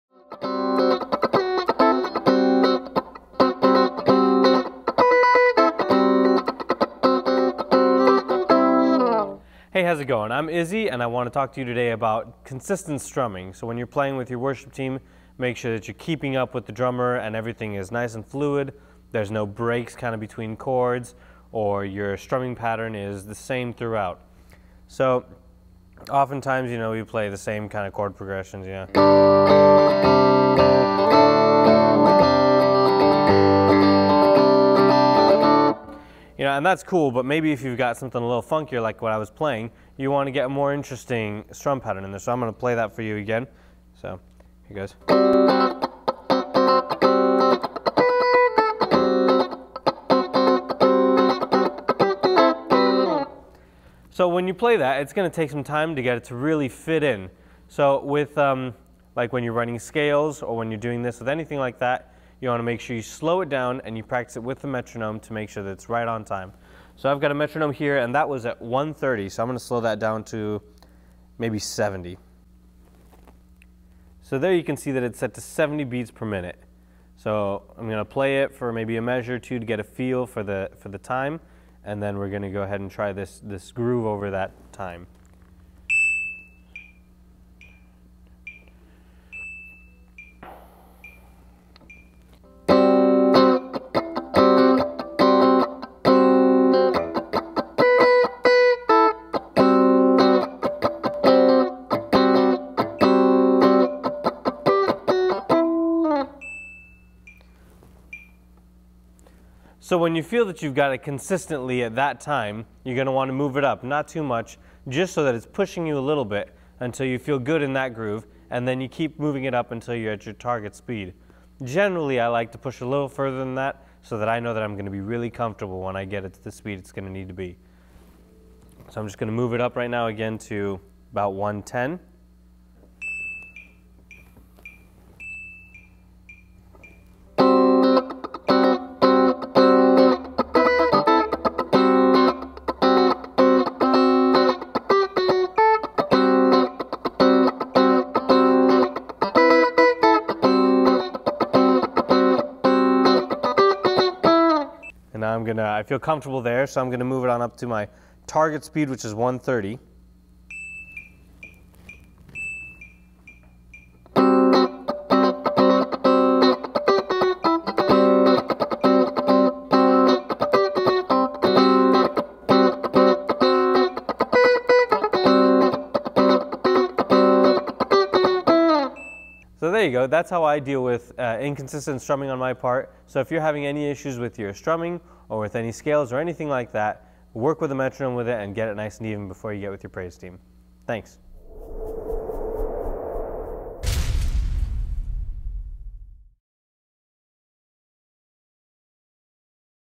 Guitar Technique Tutorial: Consistent Strumming
You've learned about pedals, now learn some technique! Here Izzy breaks down a few practice tips to improve you ability to blend with and compliment your band/worship team.